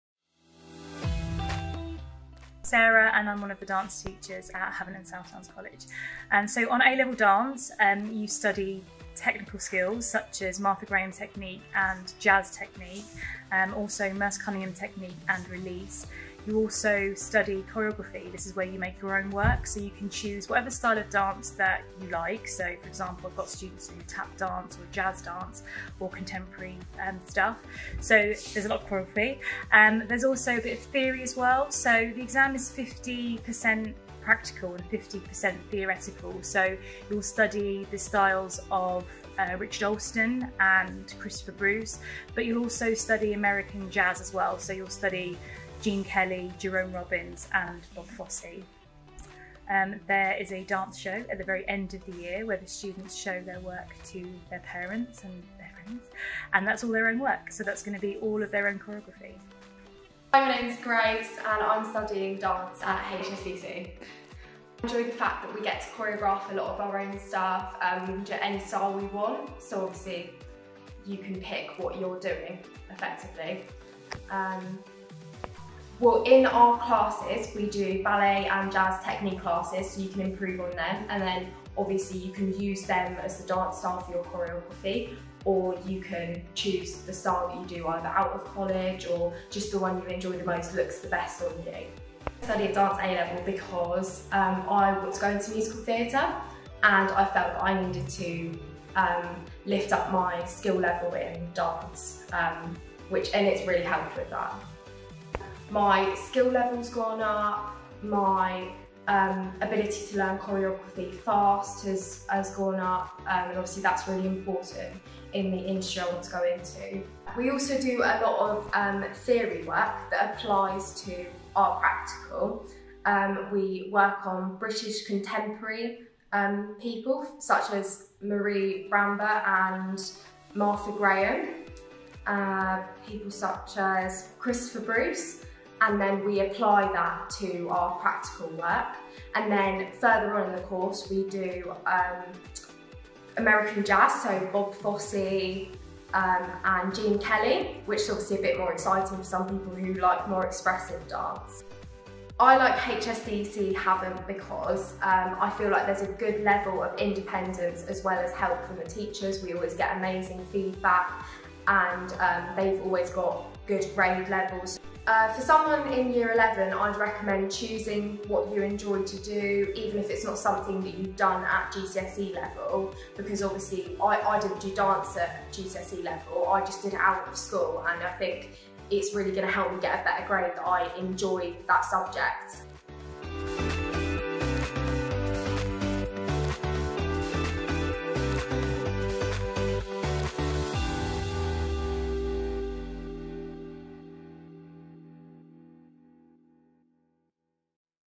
Dance A Level (HSDC Havant)
Find out more about Dance A Level at Havant Campus, as part of our online open event.